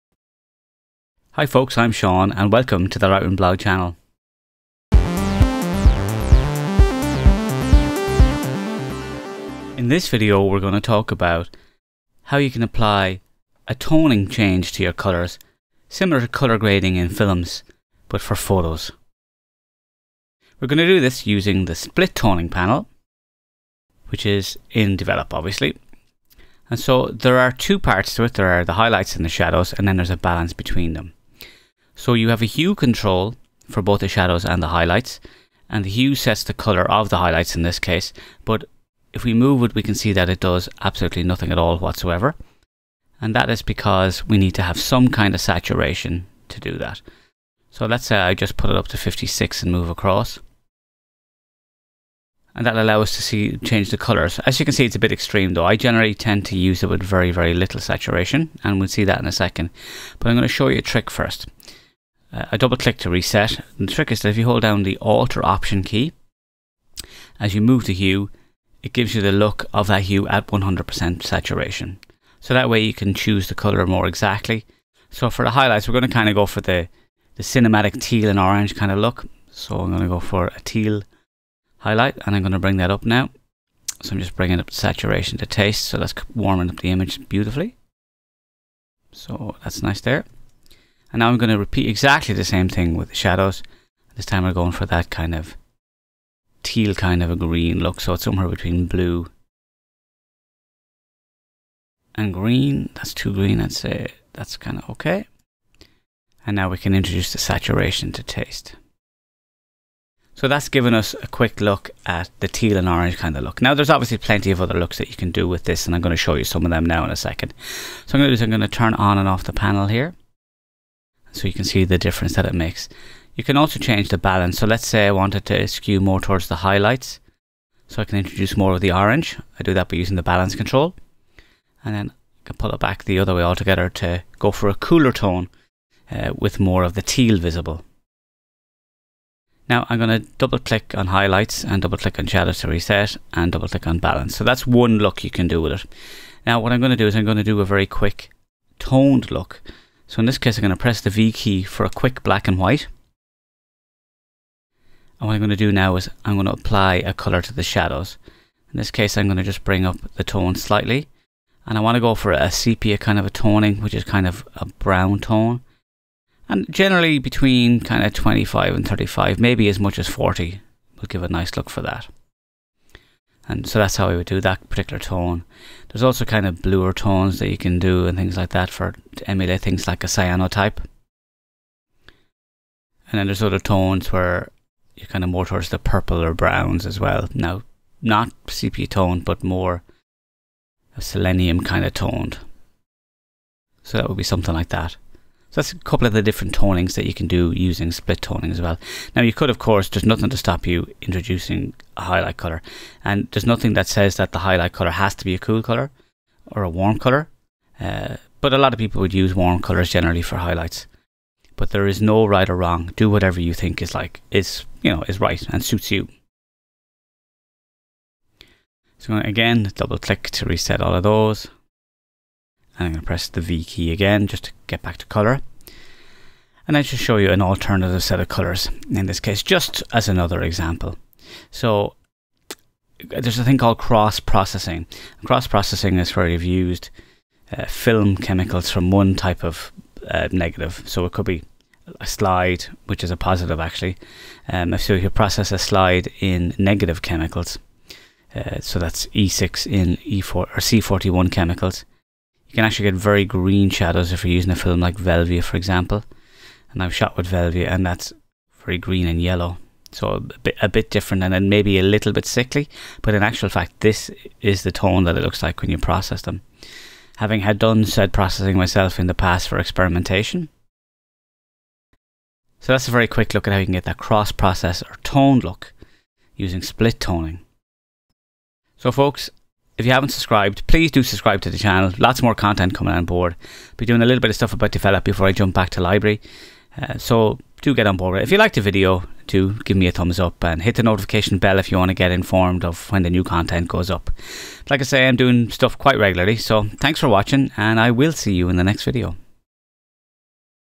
Grade Your Photos With Split Toning in Lightroom
Hey Lightroomies, first of a few Develop videos for you now. This time it's all about toning your images. It's a processing that's quite similar to how films are graded giving them their mood.

If you want quick and easy image grading, check out my LRB Serenity Presets

My Book! The Indispensable Guide to Lightroom CC